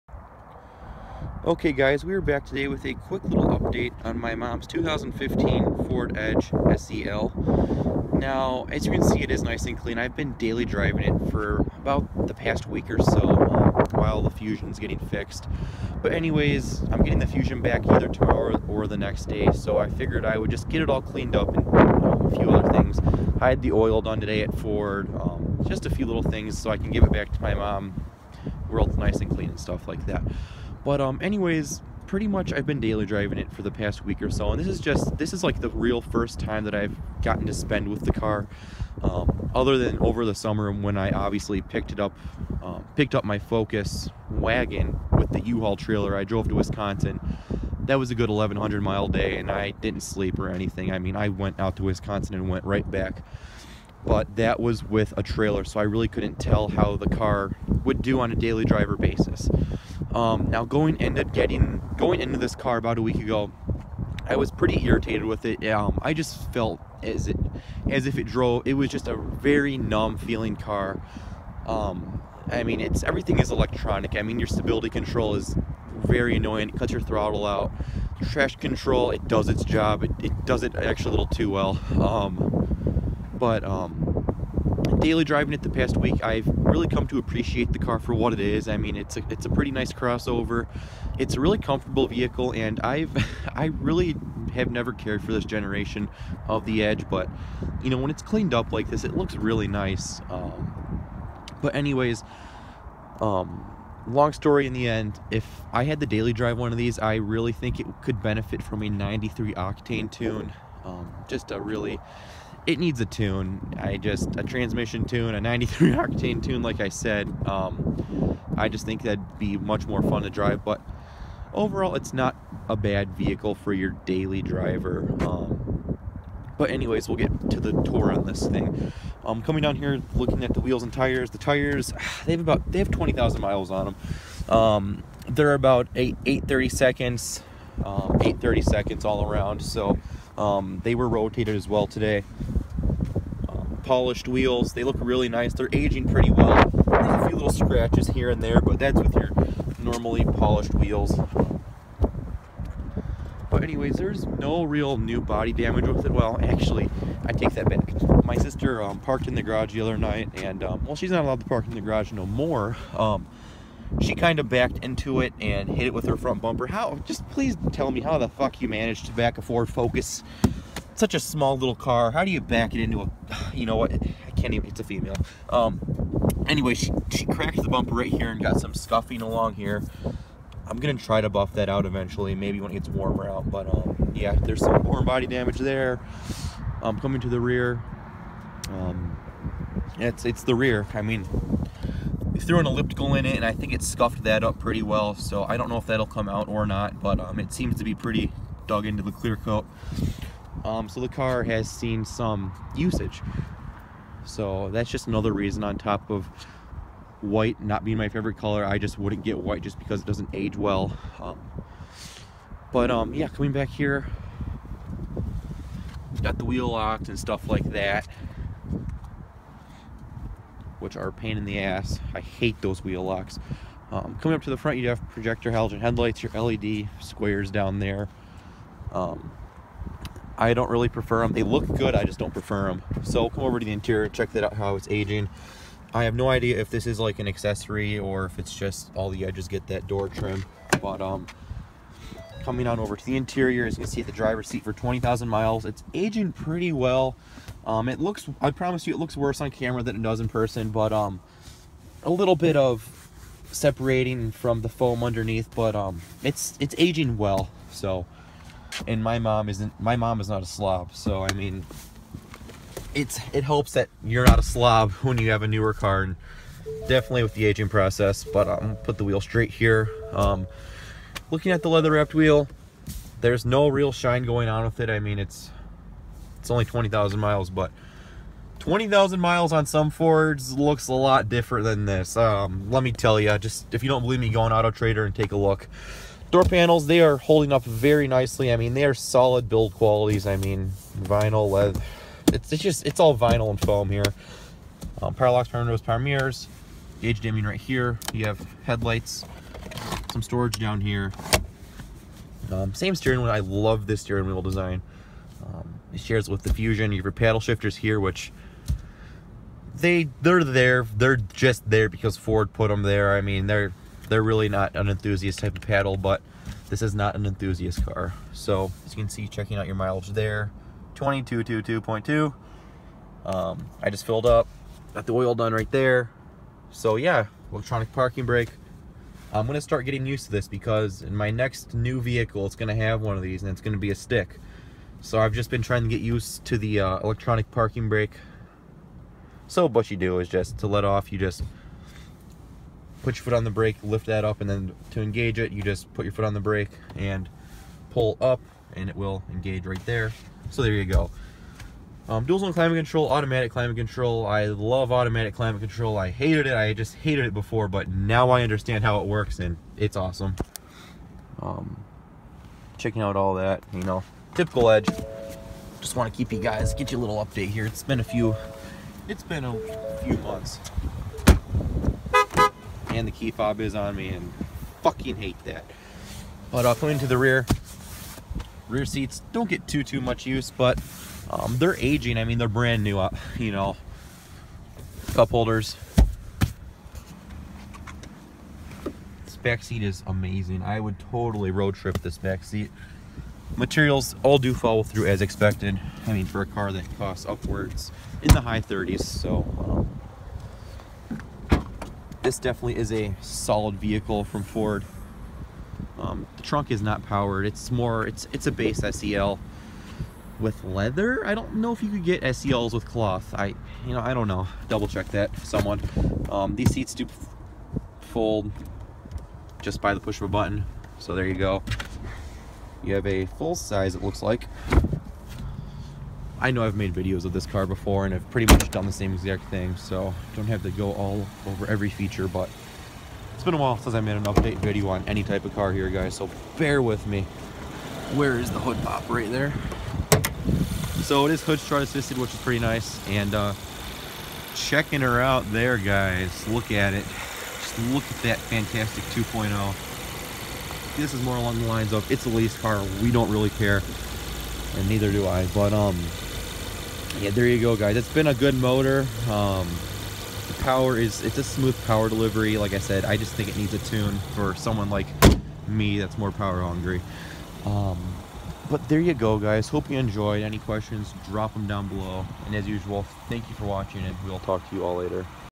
2015 Ford Edge SEL 20,000 Mile Update
So far it's proven to be one of the better vehicles we've owned!